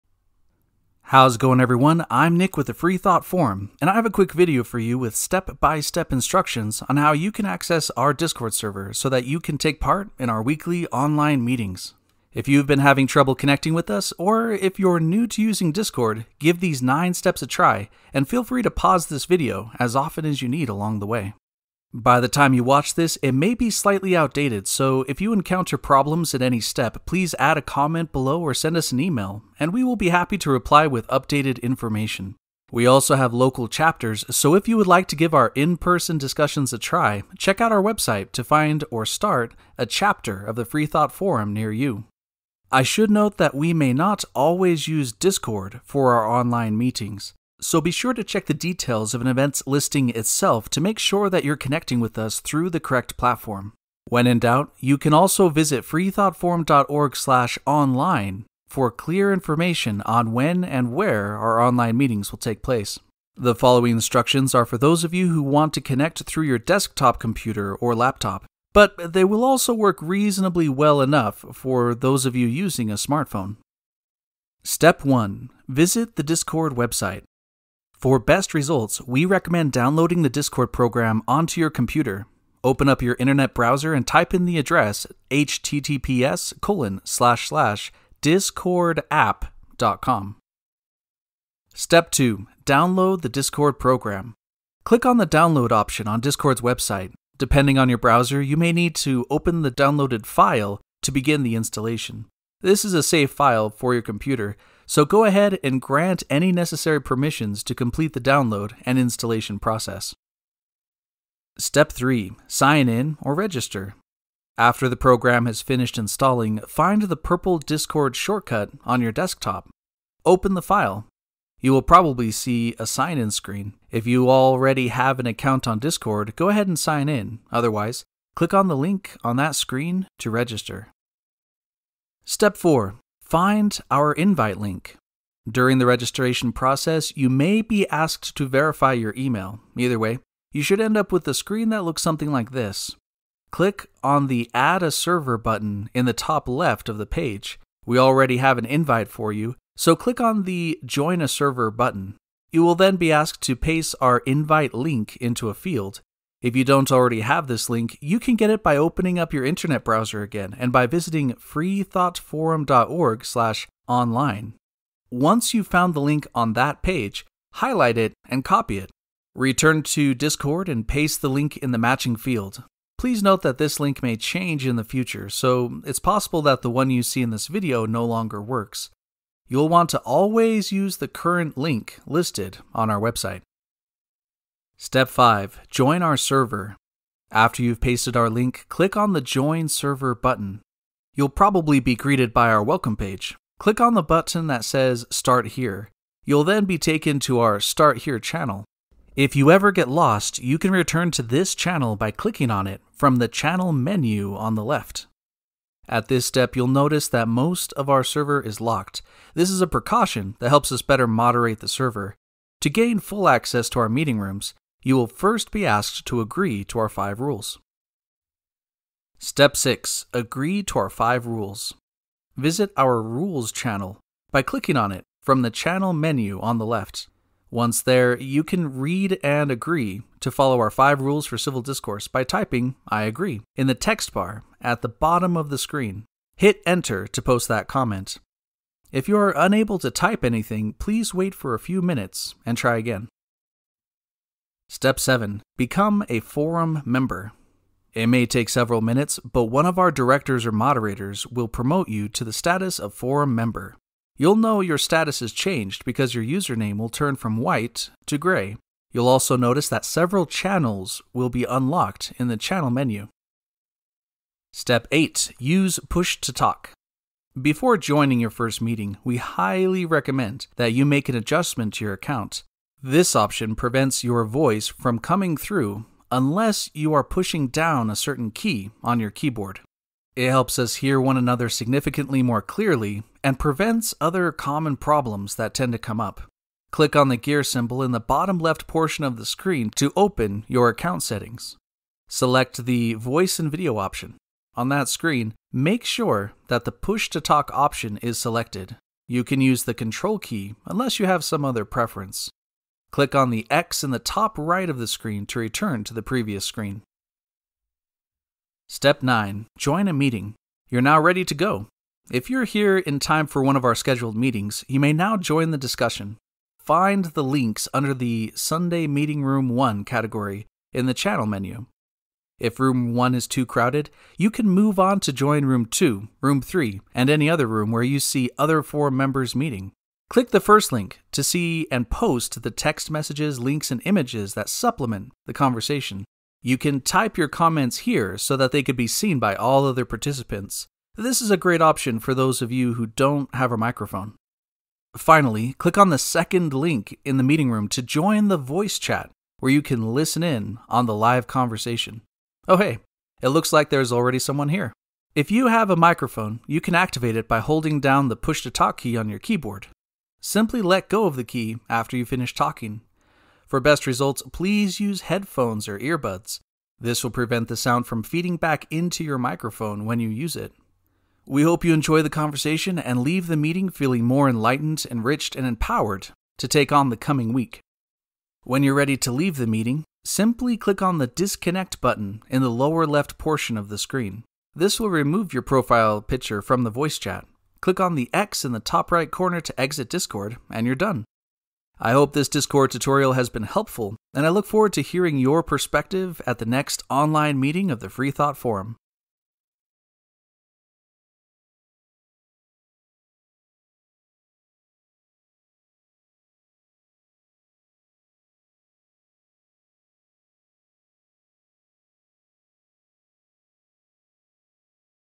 How to Meet with us on Discord • Using the Discord App for Online Meetings, a Step by Step Tutorial
How's it going, everyone? I'm Nick with the Free Thought Forum, and I have a quick video for you with step-by-step instructions on how you can access our Discord server so that you can take part in our weekly online meetings! If you've been having trouble connecting with us, or if you're new to using Discord, give these nine steps a try and feel free to pause this video as often as you need along the way.

Written and Directed by Nicholas Burk